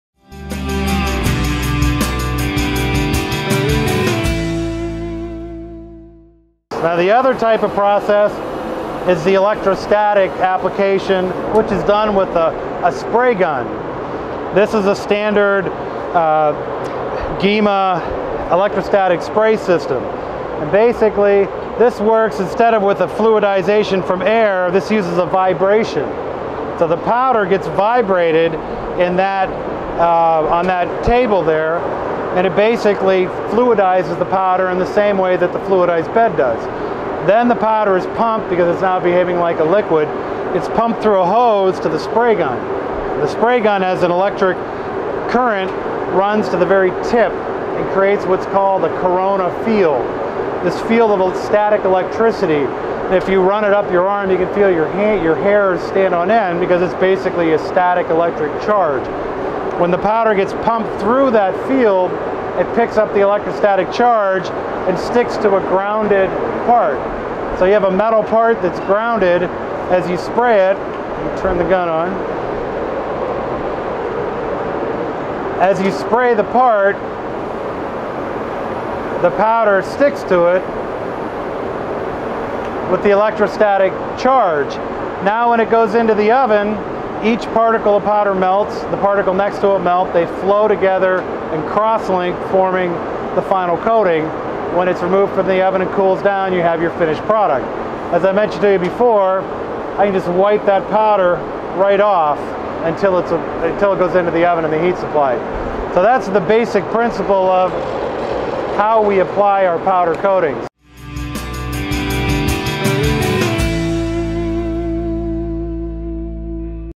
Powder Coating: Electro Static Spray Application
Advanced Finishing USA CEO & Powder Coating Authority, Greg Yahn describes the Electro-static Spray Application process used in powder coating surfaces.